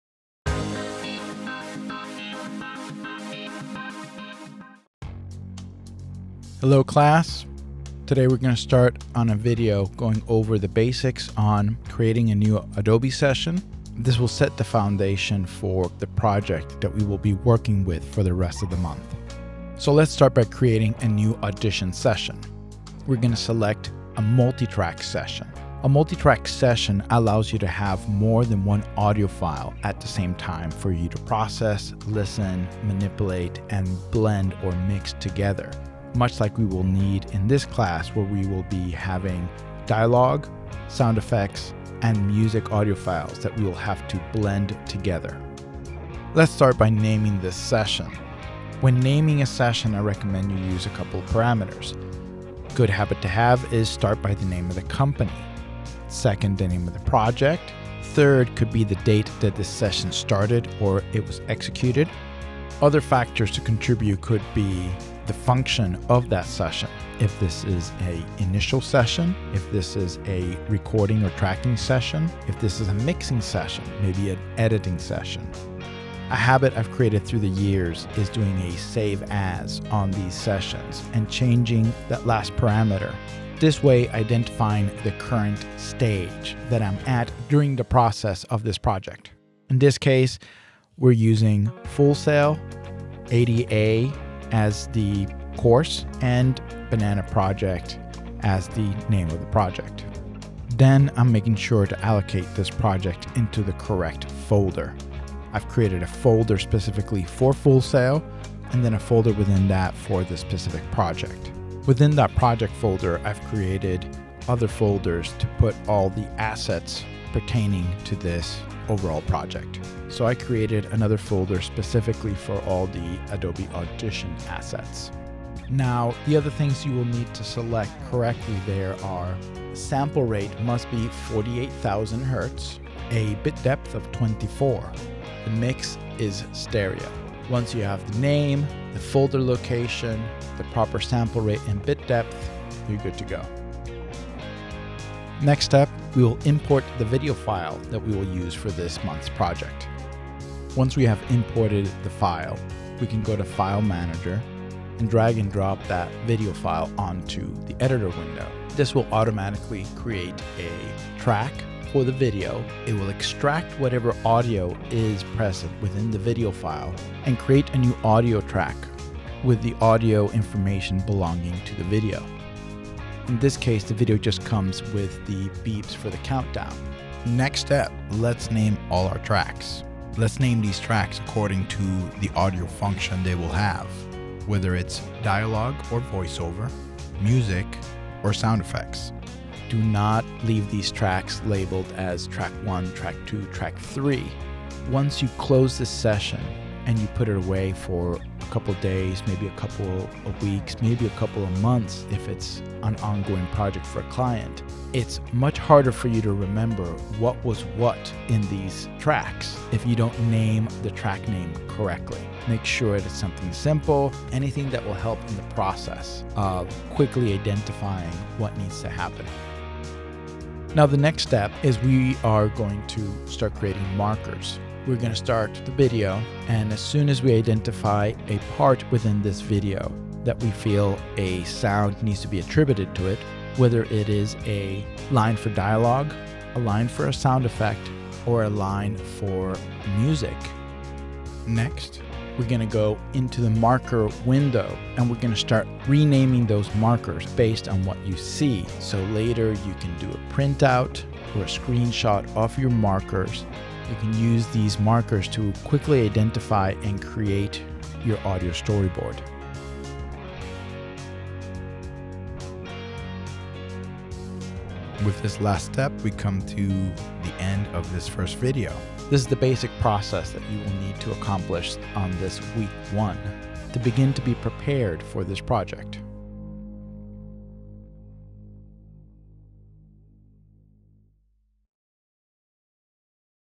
MCBS ADA Project- Audition Session Setup
This is the first of a series of tutorials that I have made to walk your through this months project. Watch this video to learn where to begin working on your project in Audition.